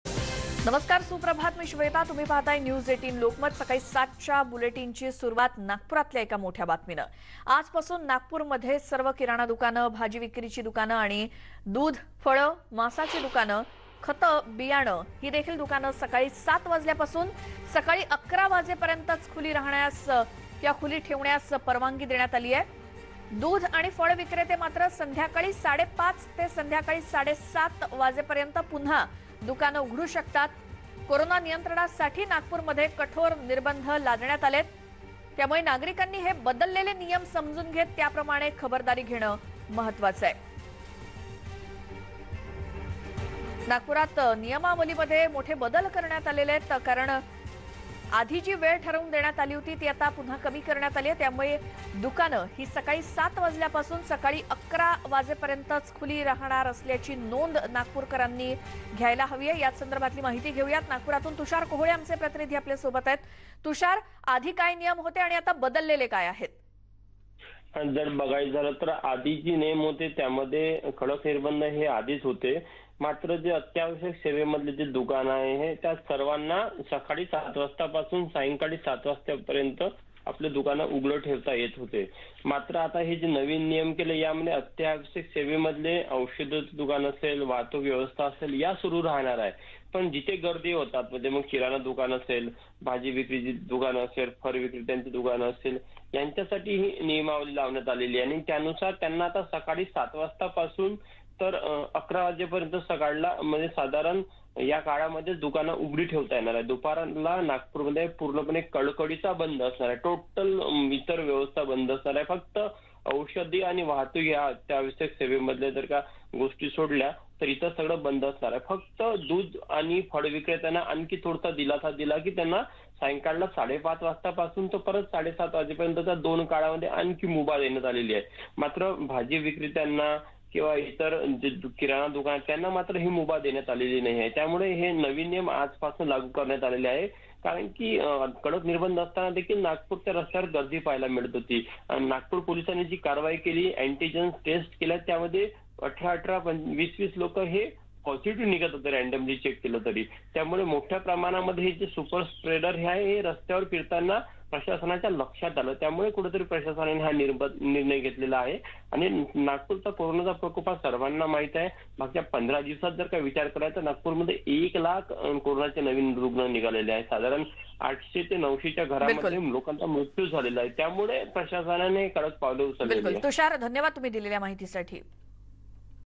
Nagpur : नागपुरात नियमावलीत मोठे बदल, सकाळी 7 ते 11 दरम्यान दुकानं रहाणार खुली
राज्यात वाढत्या रुग्णसंख्येमुळे चिंता वाढली आहे. राज्यात संचारबंदी लागू करण्यात आली असली तरी रुग्णसंख्या कमी होत असल्याचं दिसत नाही. त्यामुळे येत्या काही दिवसात लॉकडाऊन कडक करण्याचे संकेत देण्यात आले आहेत. दरम्यान राज्यात एक मोठा निर्णय घेण्यात आला आहे. राज्यातील किराणा दुकानं (Grocery store) मर्यादित वेळे पुरत्याच खुली करता येणार आहेत. सकाळी 7 ते 11 या वेळेत किराणा दुकानं सुरू राहतील